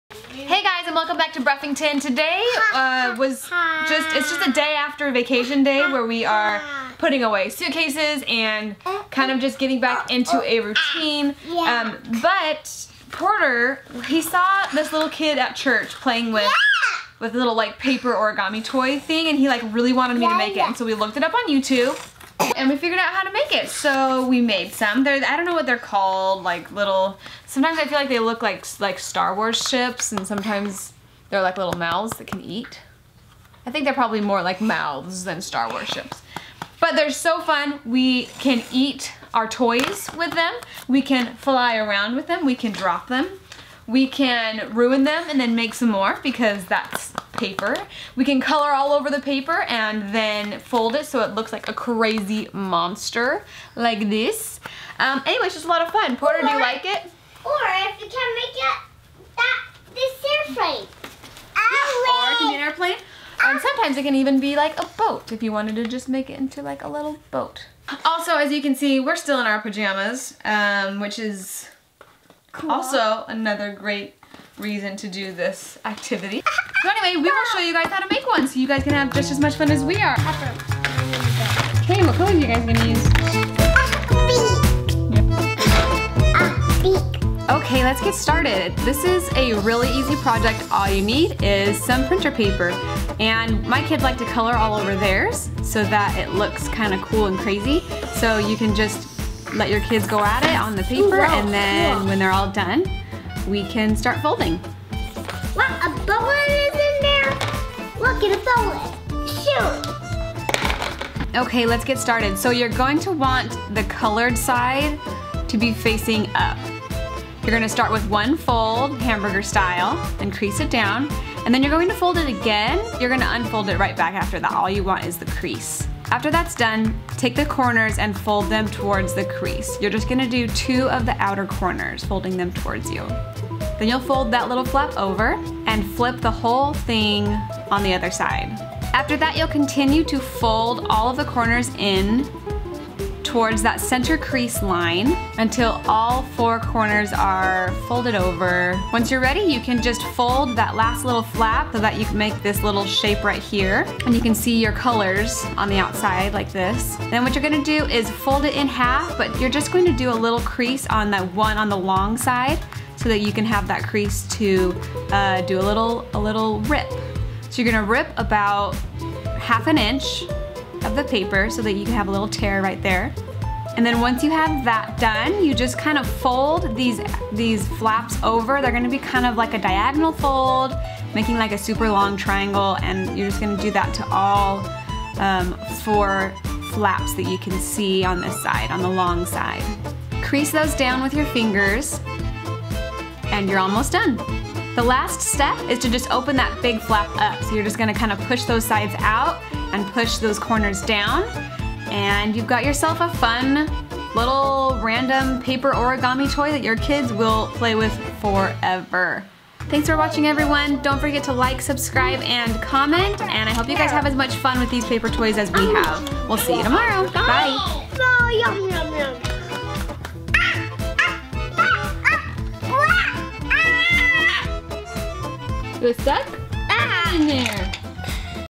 Boredom Buster // How To Make An Origami Snapper
The other day at church Porter saw the family in the row next to us playing with these fun origami snapper-things! He said he wanted to make one, so I learned how, and thought it would make for a good video today! My kids LOVE these, and it has been a great alternative to putting them in front of the TV/iPad when they are needing something to do! Enjoy!

Tara’s Links

Zach’s Links